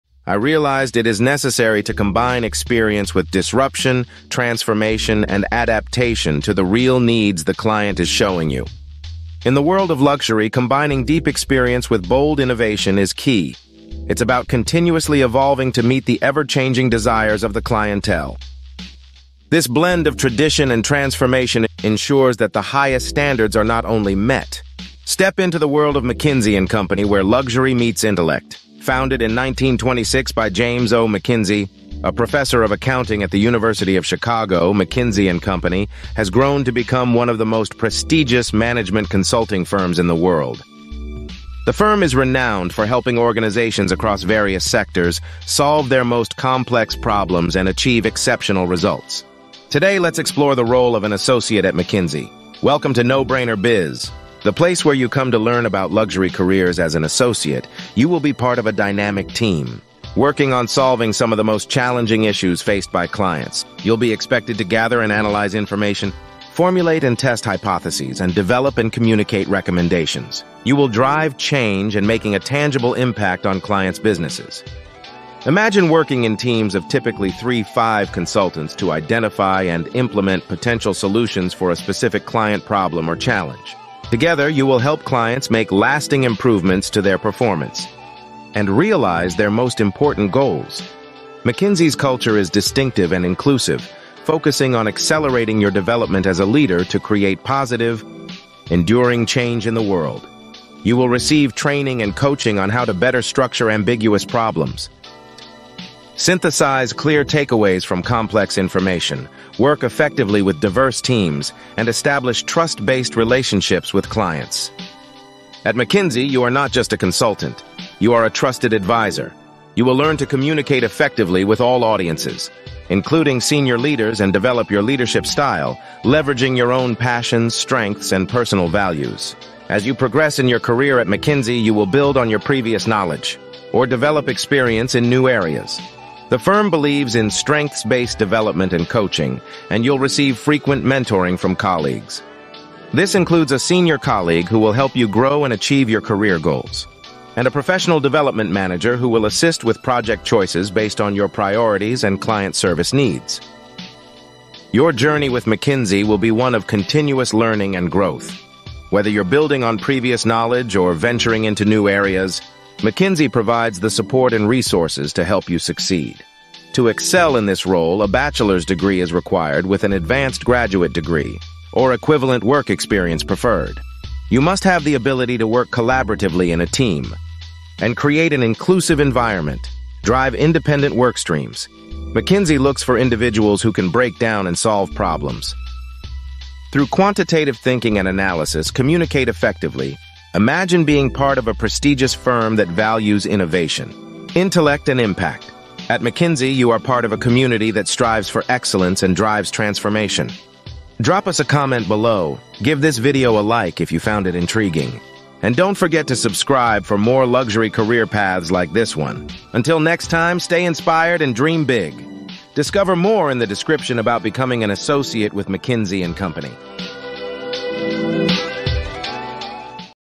Step into Luxury: Business Consultants make $100K to $1M
In this video, we share the luxurious career path of an Associate at McKinsey & Company. Discover the roles, responsibilities, and the unique blend of tradition and transformation that defines this prestigious firm. Explore how you can become part of a team that drives innovation and impact across the globe.

Discover more about becoming a McKinsey & Company Business Consultant